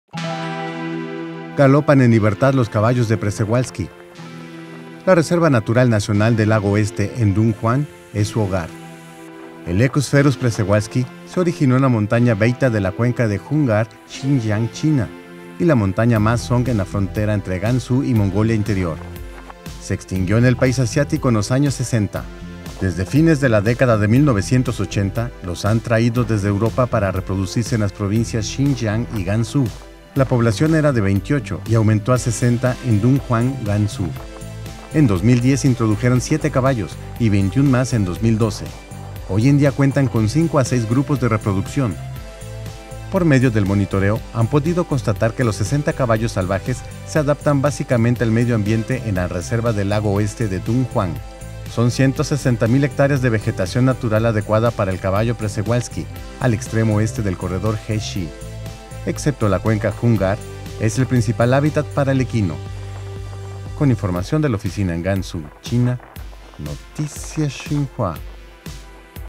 Se expande población de caballos de Przewalski en China
Galopan en libertad los caballos de Przewalski. La Reserva Natural Nacional del Lago del Oeste en Dunhuang es su hogar.